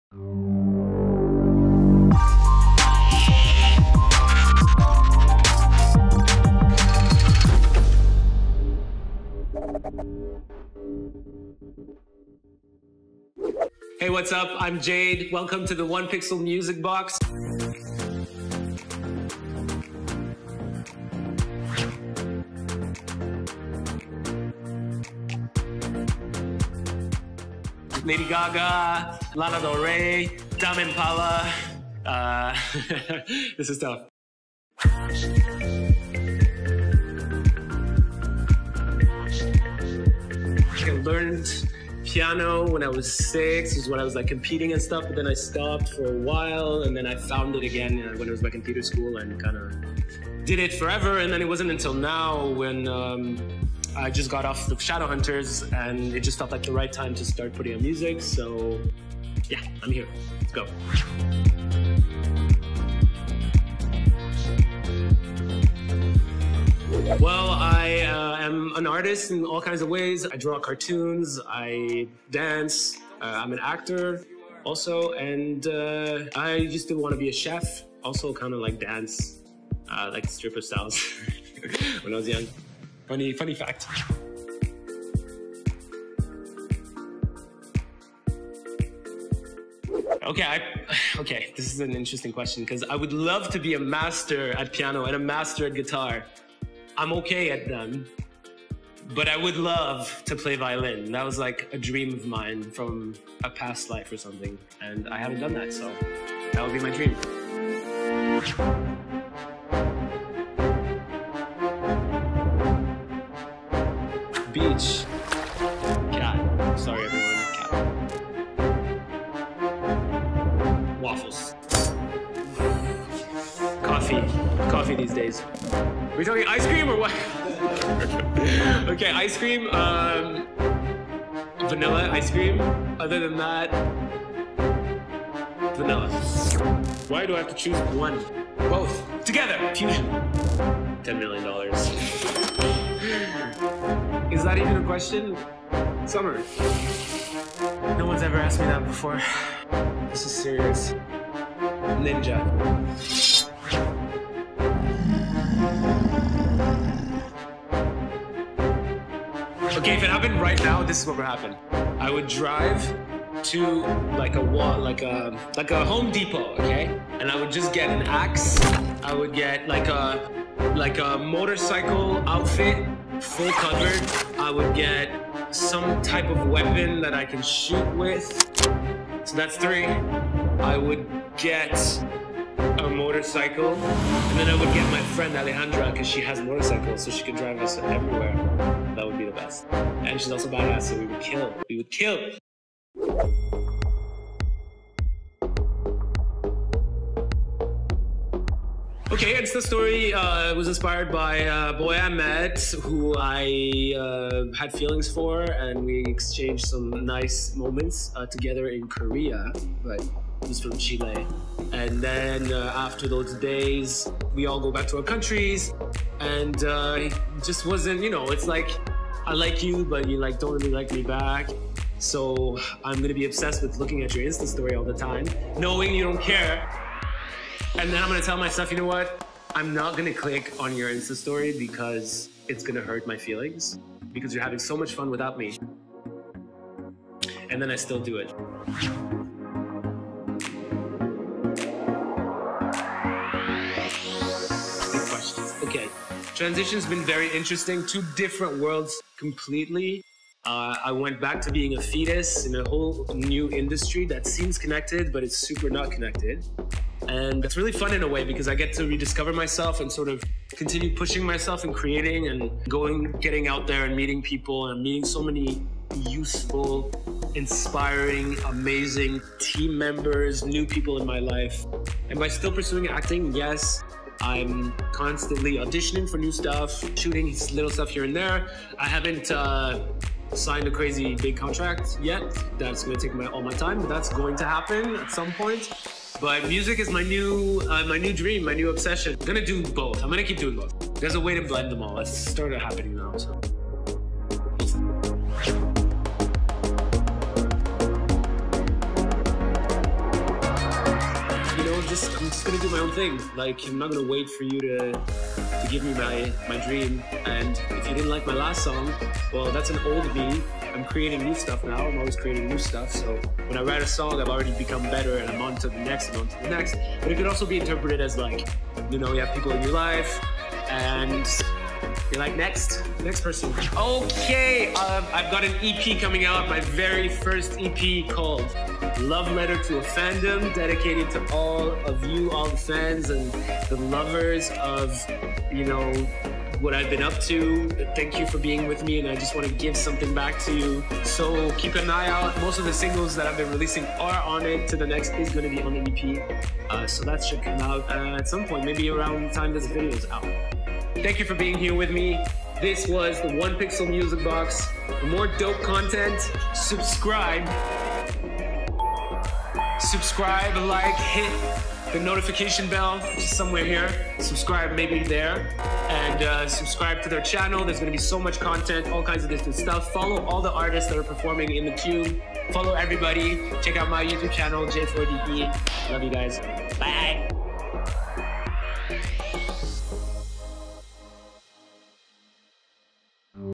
THE MUSIC BOX | Who is J4DE | EP.6
1PXL Studio presents “The Music Box”: a new weekly web series showcasing Montreal’s up and coming musical talents.

Artist: J4DE

Produced by Chady Ghorayeb, Dylan Gouze, Jonathan Della Vecchia
Creative Director: Chady Ghorayeb
Lighting Design: Nicolas Laurin and Tommy Raymond

Directors of photography: Chady Ghorayeb, Jonathan Della Vecchia
Camera Operators: Chady Ghorayeb, Jonathan Della Vecchia, Amber Bellemare
Audio Engineer: Dylan Gouze
Editor: Jonathan Della Vecchia
Colorist: Chady Ghorayeb
Motion Graphics: Chady Ghorayeb
Production Assistants: Lara Debay, Roberto Yermo, Mark Doummar
Youtube consultant: Alexandre Bachaalani

Sound mixing and editing: DLN